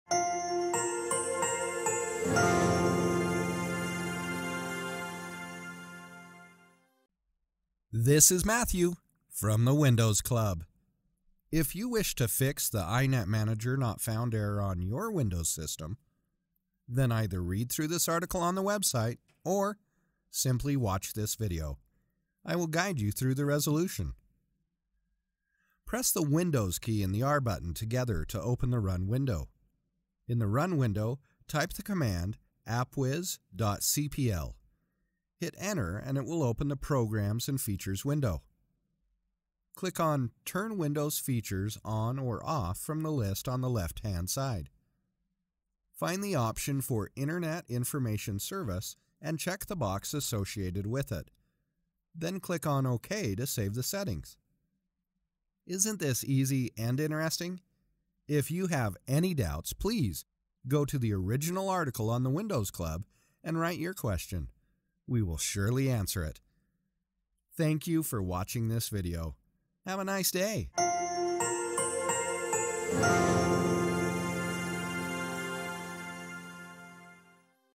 Fix inetmgr not found on Windows 11/10
In this guide, we will help you fix the issue where you may see a Windows cannot find inetmgr error message. InetMgr.exe is an executable file to launch the Internet Information Services (IIS) Manager application in Windows 11/10. You can use the Search box or Run Command box (using Win+R hotkey) and type inetmgr command to open the IIS Manager window. However, some users have reported that such command is not working on their Windows computers because either the InetMgr.exe file is missing or corrupted.

Here are a few fixes and suggestions:

1] Enable IIS Manager using Windows Features

2] Enable IIS Manager using Windows PowerShell

3] Temporarily disable the antivirus program

Timecodes:
0:00 Intro
0:25 Procedure through Control Panel